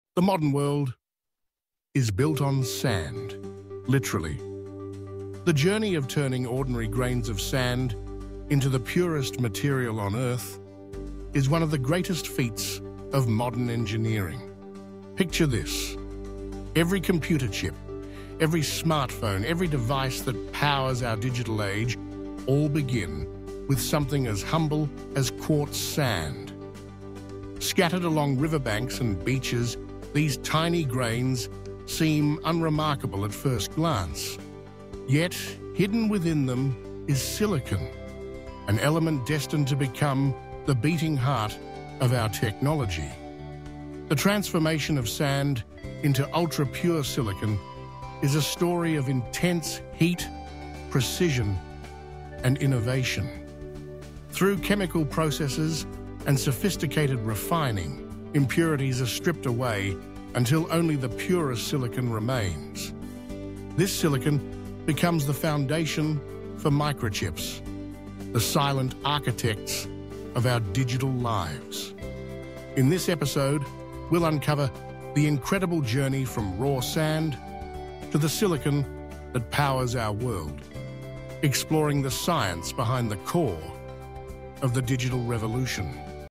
#21. The Microchip (Part 1), The Raw Material of the Digital Age
Every computer chip, every smartphone, begins with something humble: sand. This episode traces the incredible transformation of quartz sand into ultra-pure silicon, the physical foundation of the digital revolution.